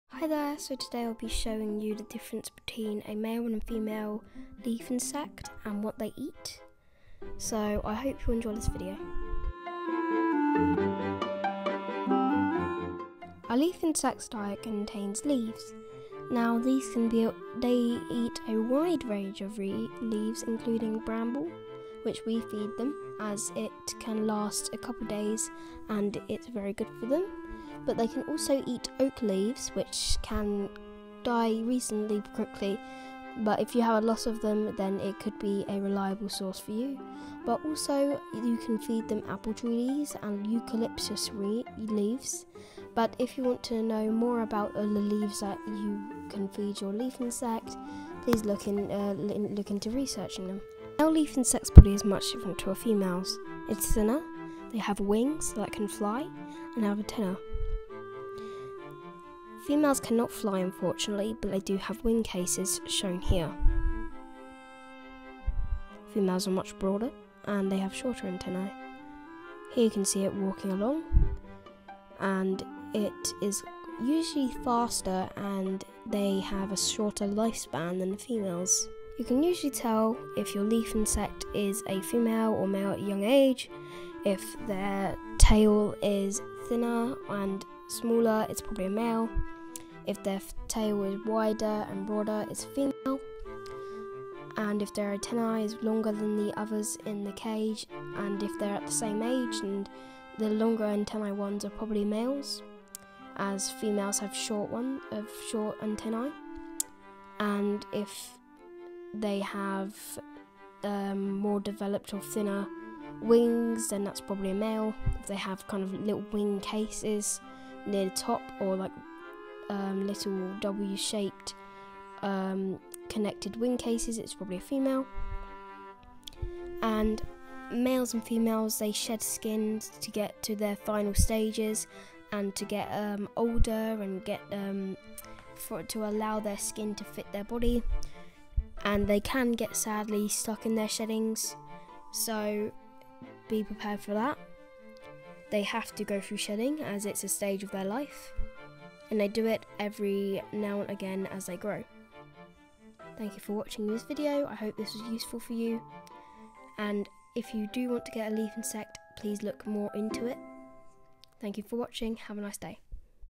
Leaf insect (phyliidae) facts!
Hello :)
If you would like to own a leaf insect, I suggest you look into it more, since these are just basic facts or things you need to know about leaf insects
If you would like more content about stick insects, please leave a comment to let me know :)